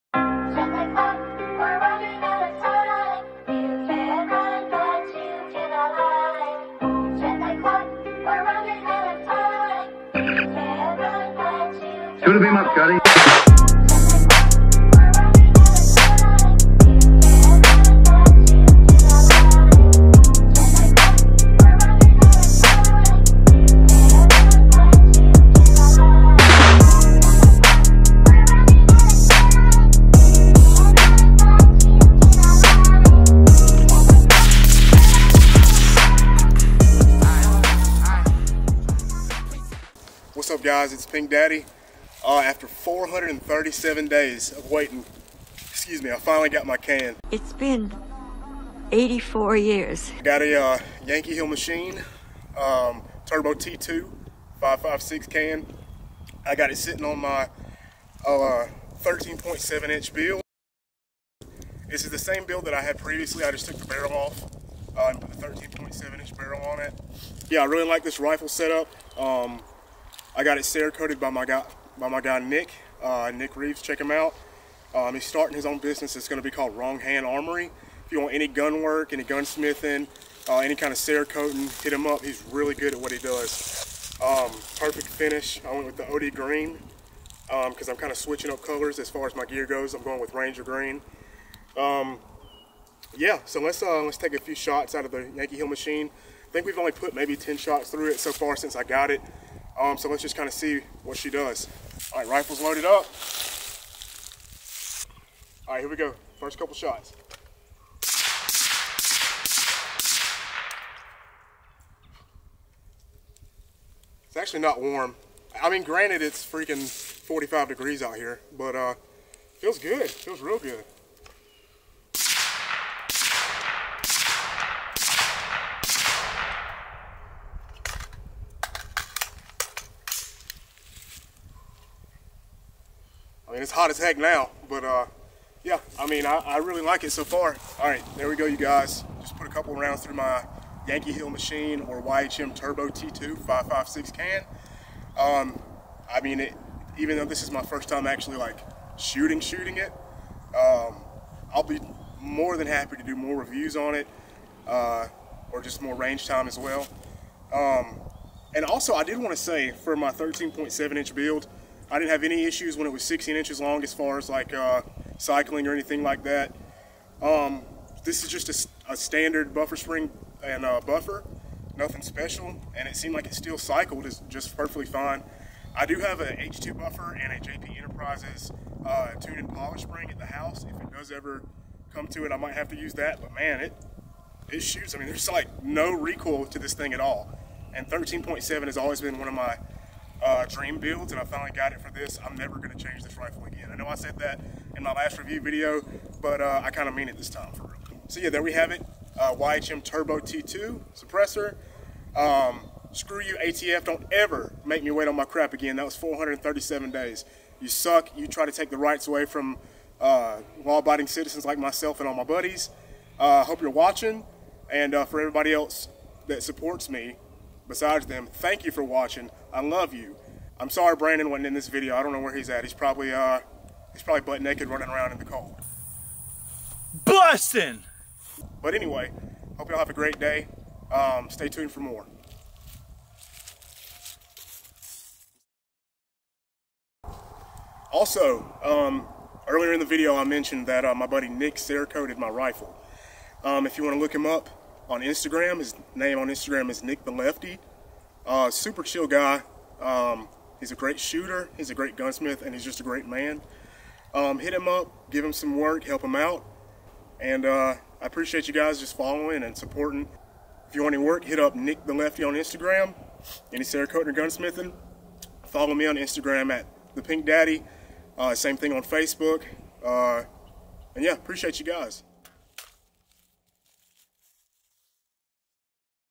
YHM TURBO T2 5.56! FIRST LOOK!
Taking a brief look at my Yankee Hill Machine Turbo T2 5.56 Suppressor!

After waiting 437 days (thanks AFT), I finally received the can via Silencer Central. This is just a brief overview of it in addition to showing my updated rifle build! I'm also going to do a complete review once I put more rounds through it.